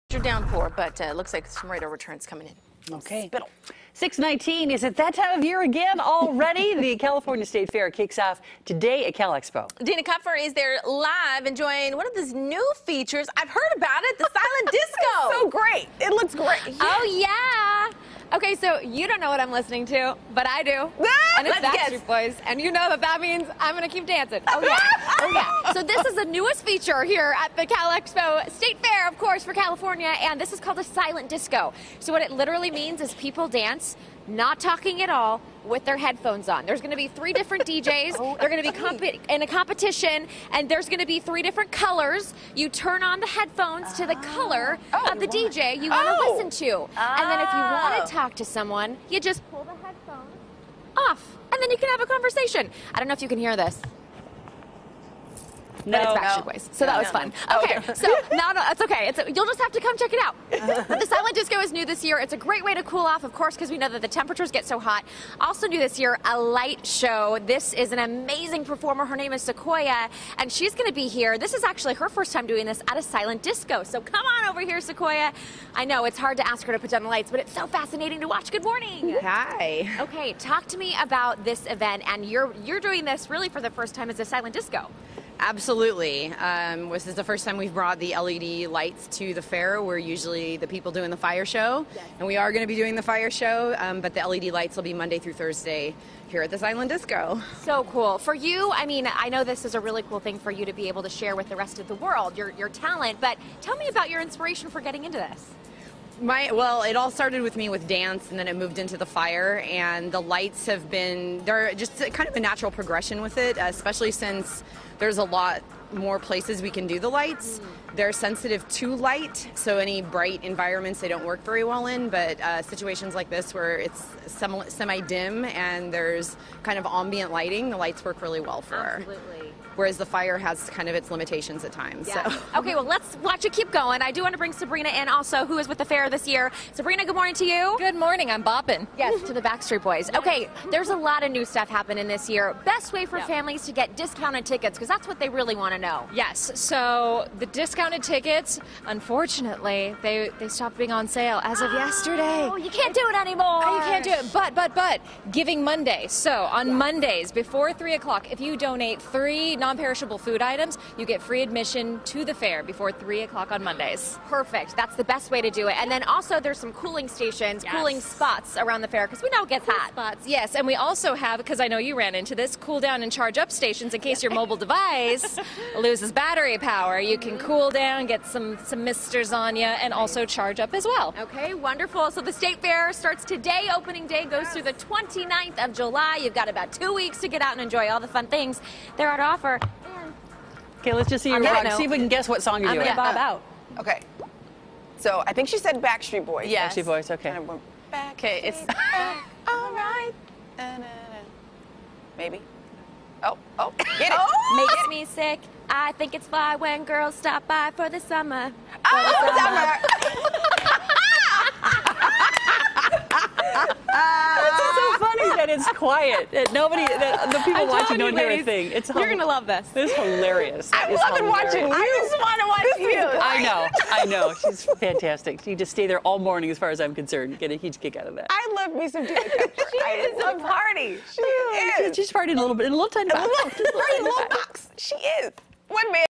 First Day of State Fair Pt. 3
Dina is back at the California State Fair dancing and learning about the Silent Disco!